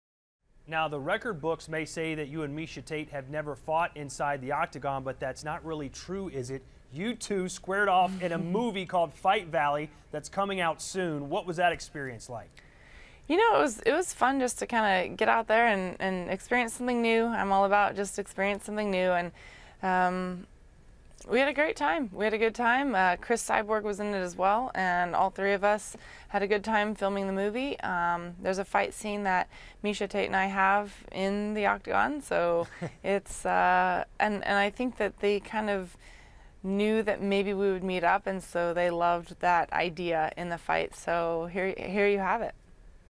Holm talks movie fight scene vs. Tate
UFC women's bantamweight champion Holly Holm (10-0) faces Miesha Tate (17-5) at UFC 196, but March 5 won't be the first time they've met in a cage.